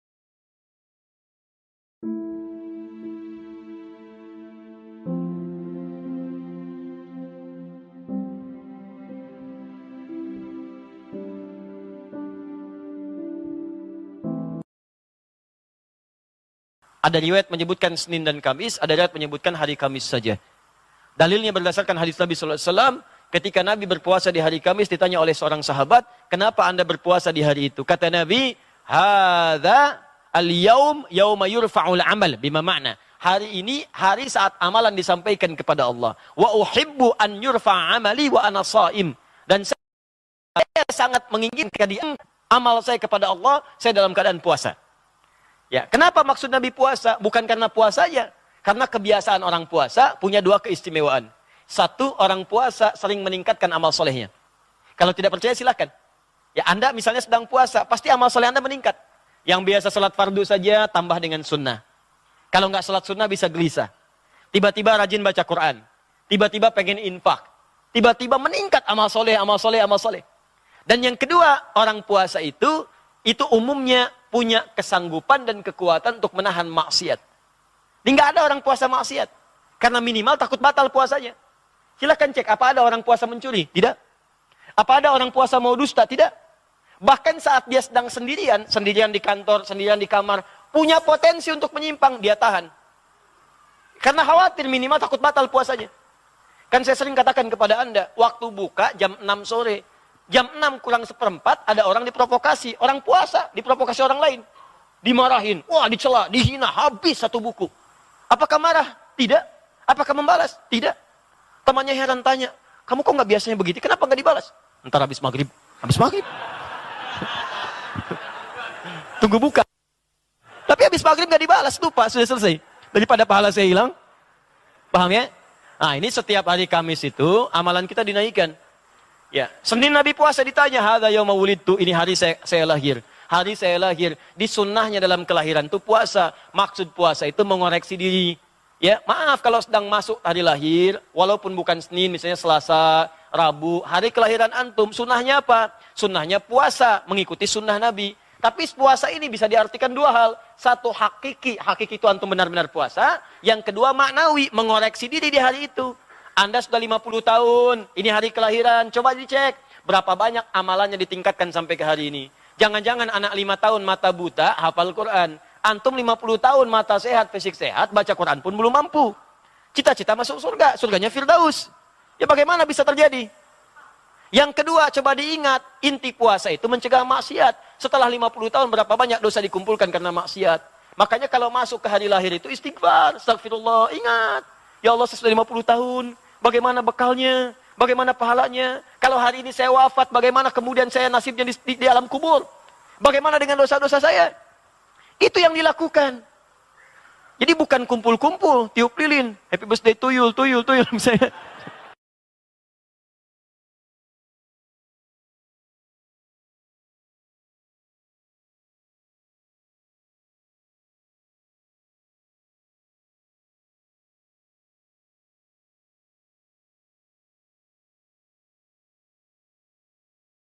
RASULULLAH PUASA SENIN - KAMIS!!!DENGAN MANFAAT MANFAATNYA
Dalam melakukan puasa Senin Kamis, biasanya menjalankan aktivitas seperti berpuasa sepanjang hari, beribadah, dan menjauhi makan dan minum serta tingkah laku yang tidak baik. Puasa ini juga merupakan momentum untuk membersihkan diri dari dosa dan menjauhkan diri dari godaan duniawi. Puasa senin kamis juga di lakukan oleh baginda Rasulullah SAW, dan kita sebagai umat Nya disunahkan melakukan hal yang sama.

semoga kita selalu mendapatkan perlindungan dari Allah SWT dan mendapatkan syafaat dari beliau Rasulullah Muhammad SAW